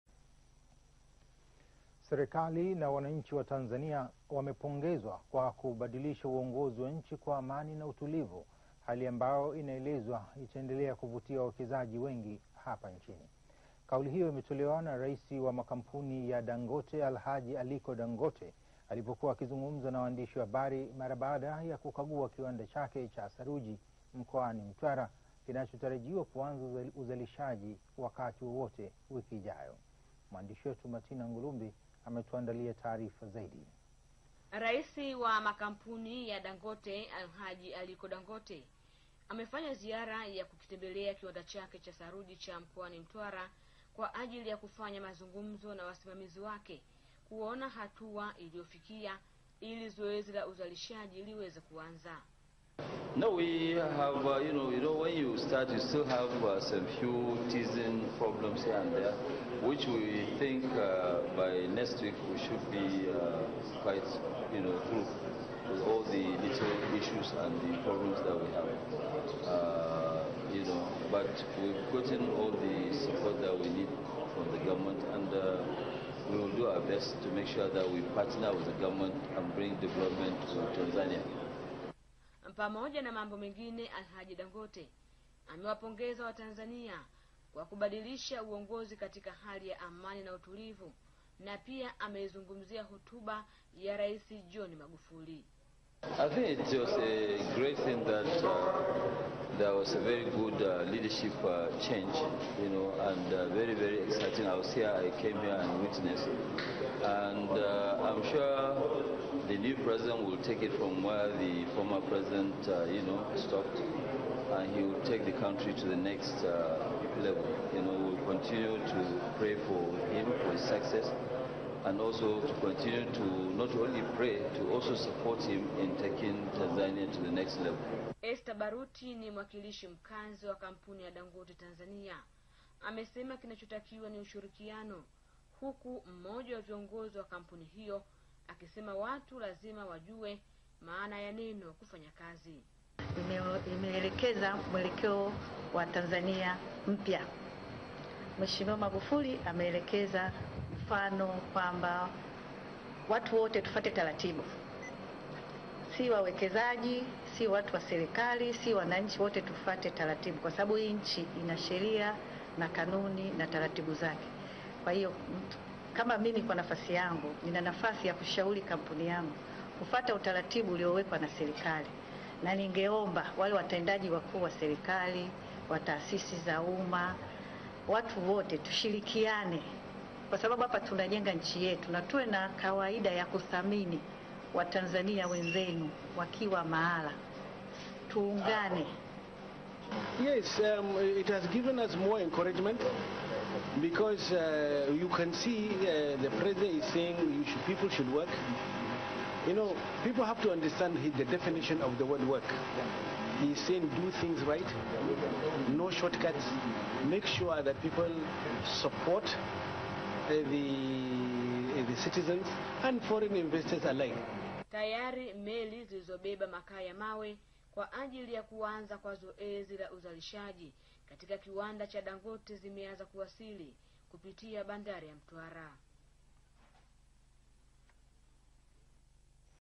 Rais Wa Dangote Aipongeza Serikali Ya Tanzania
Rais wa kampuni ya Dangote ameipongeza serikali na wananchi kwa kubadilisha serikali kwa amani jambo ambalo litazidi kuvutia wawekezaji.